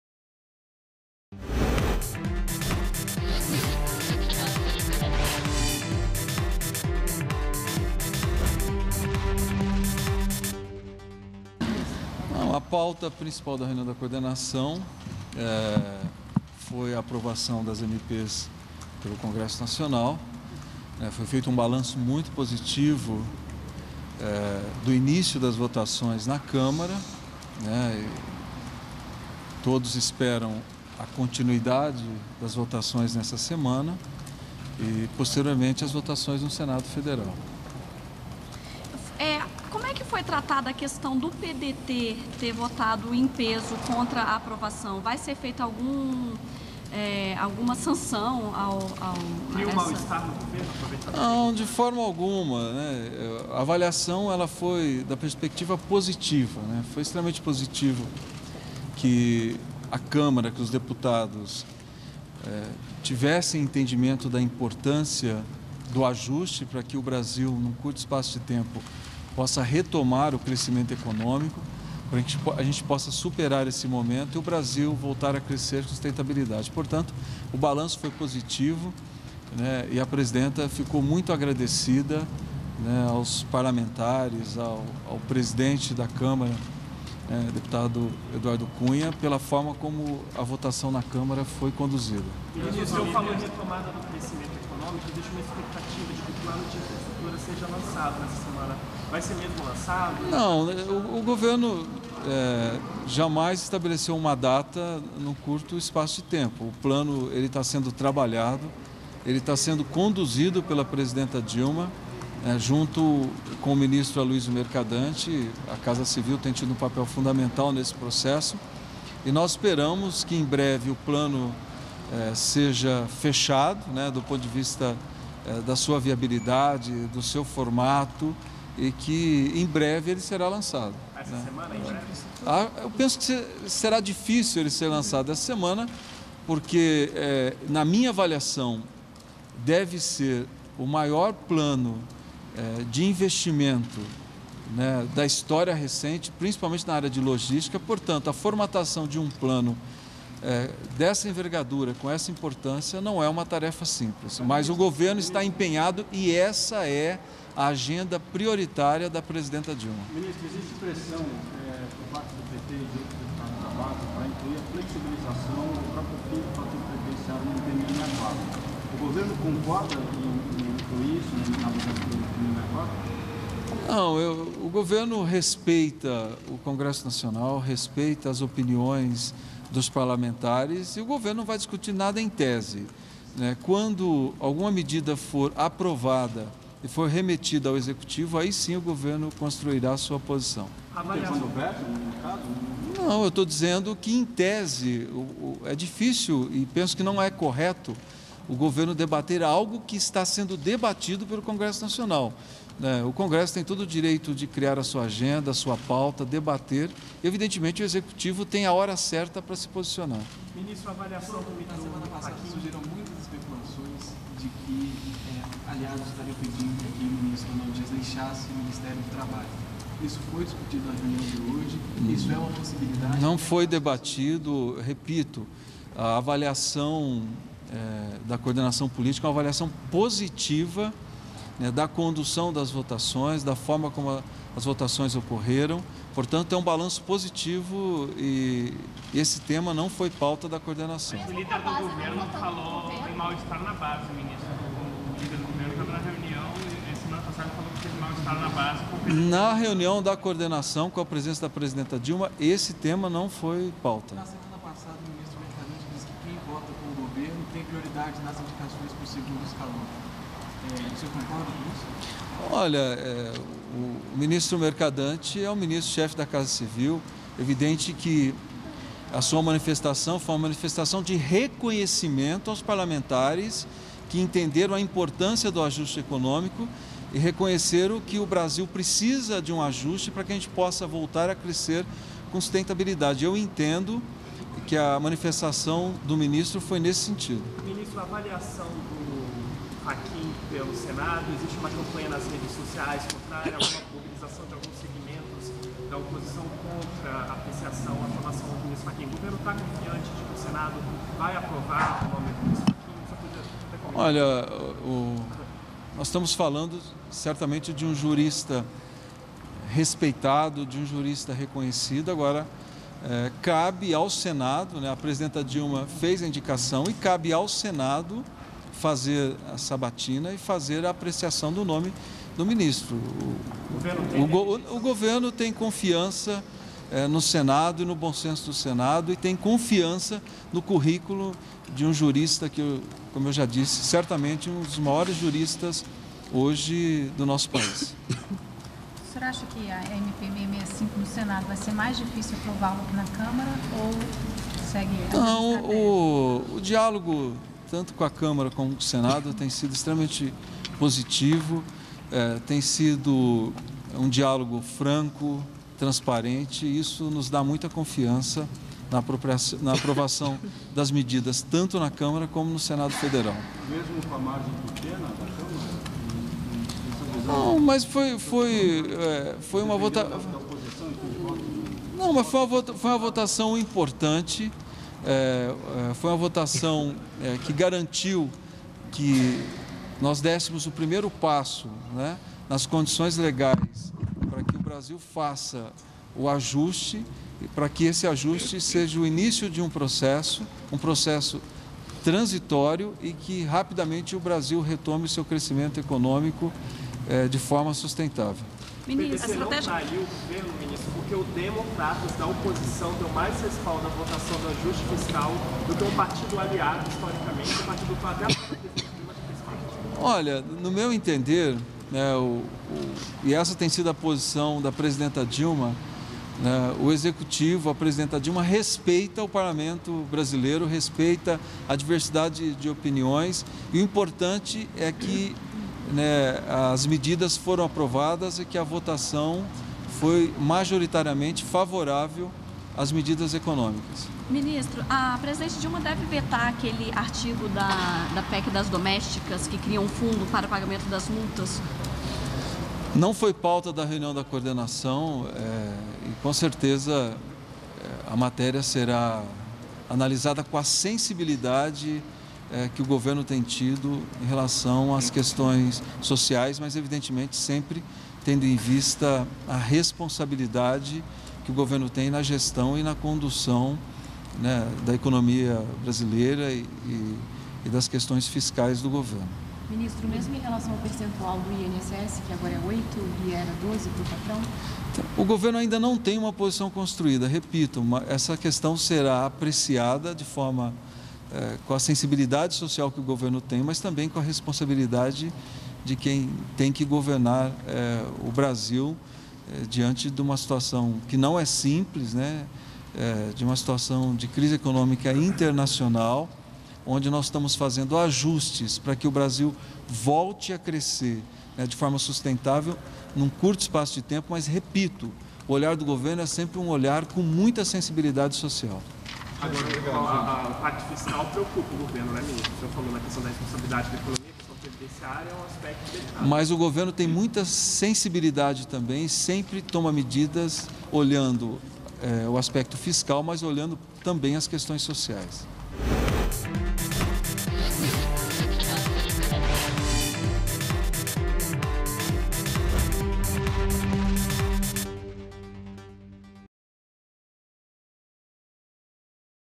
Plano de investimentos é agenda prioritária para o governo, ressalta ministro
NOTÍCIAS DA FONTE - 11.05.15: O ministro-chefe da Secretaria de Comunicação Social (Secom), Edinho Silva, conversou com a imprensa nesta segunda-feira (11) após reunião de coordenação no Palácio do Planalto, em Brasília. Edinho Silva disse que o governo espera a continuidade das votações das medidas provisórias 664 e 665 pelo Congresso Nacional e comentou o balanço positivo feito pela presidenta Dilma Rousseff sobre as aprovações. O ministro falou ainda sobre o plano de investimentos que deve ser lançado em breve e é agenda prioritária para o governo federal.